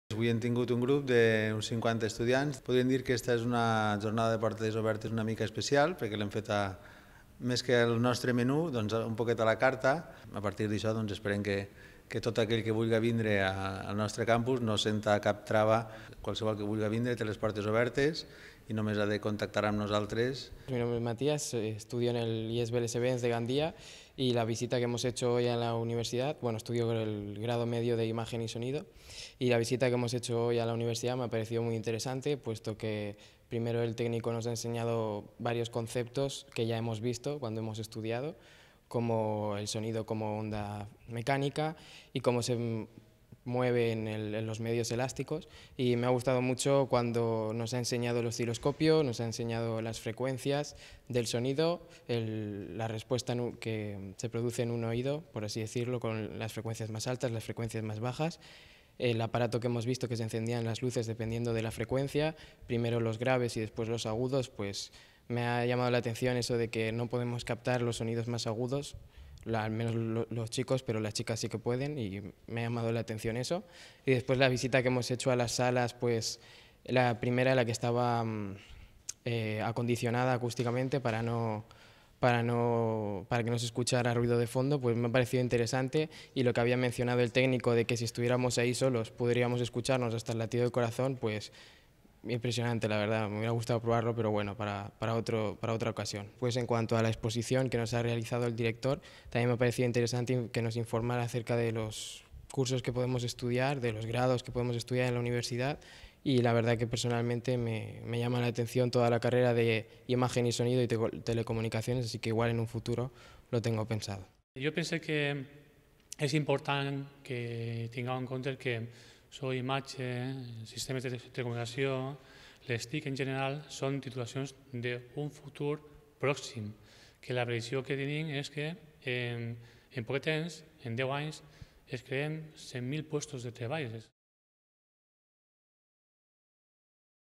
Visita a les instal·lacions de So i Imatge al Campus de Gandia de la UPV
Ens encanta que els estudiants preuniversitaris vinguen a conèixer-nos! Ací us deixem una visita de l'IES Veles e Vents de Gandia, organitzada sota demanda del professorat del Grau Mitjà en Imatge i So 🎶📡📲

El Campus oferta:

- Jornades de Portes Obertes per a centres preuniversitaris

- Jornades de Portes Obertes per a famílies

- Xarrades en instituts

I també podeu demanar visites 'a la carta'.

Benvinguts i benvingudes sempre! 😉